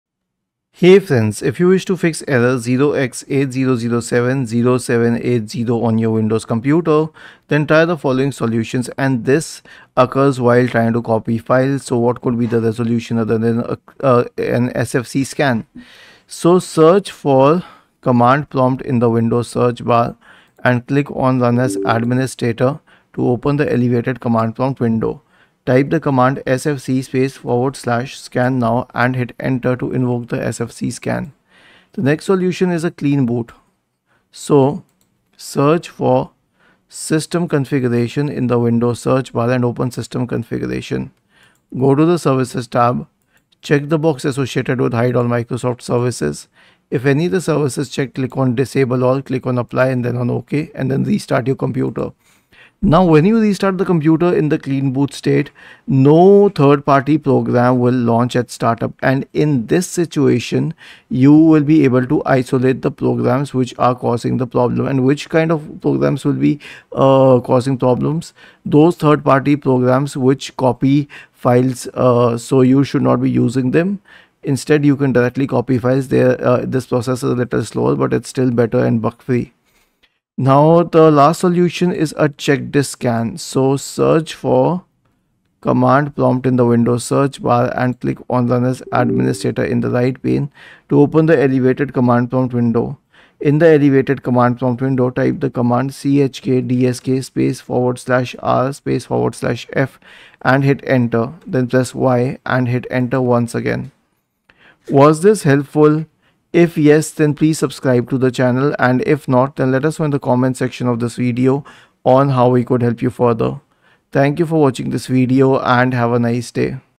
How To Fix Error 0x80070780: The File Cannot Be Accessed by the System in Windows 11
Video Title: How To Fix Error 0x80070780: The File Cannot Be Accessed by the System in Windows 11

If you wish to fix Error 0x80070780 On Windows, then please watch this video.